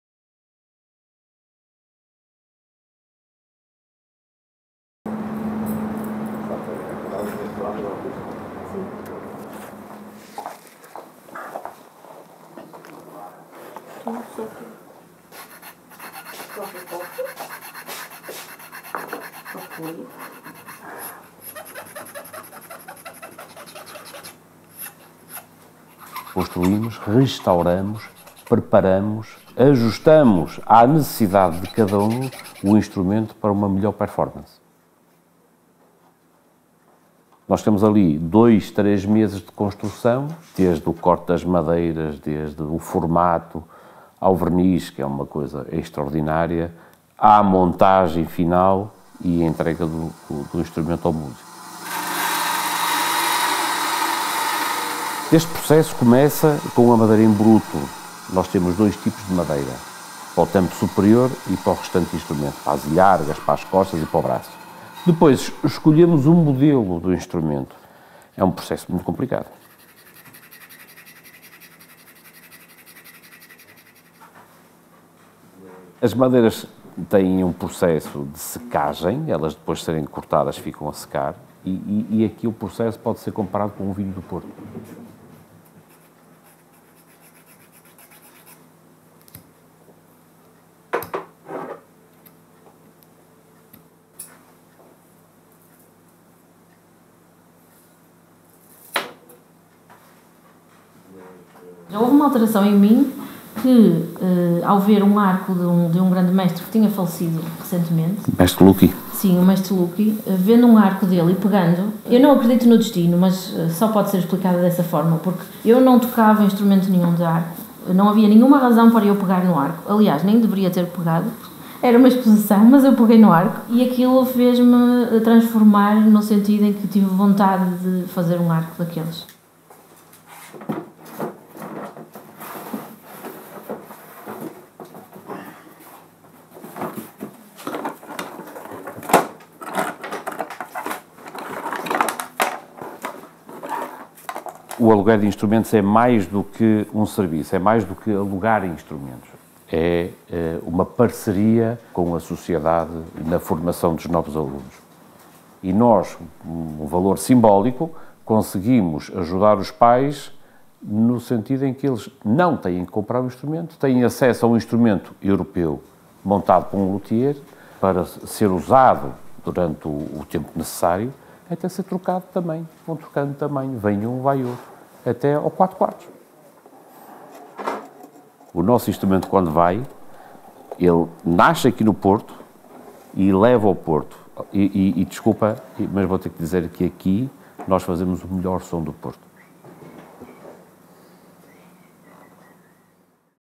Curated Porto - Filipa e Miguel Mateus
Curated Porto Apresenta: Filipa e Miguel Mateus 🎻​

​Da paixão pela música e da tradição familiar nasce a Oficina dos Violinos. Miguel Mateus o "fazedor de som" e Filipa Mateus fascinada pela construção de arcos, fundaram a Oficina dos Violinos em 2013. Cada instrumento é feito com o máximo de dedicação e construído de acordo com a necessidade de cada músico. 🎵​
Se música é umas das suas paixões ou é um curioso pela construção de violinos, não perca esta oficina! ☺️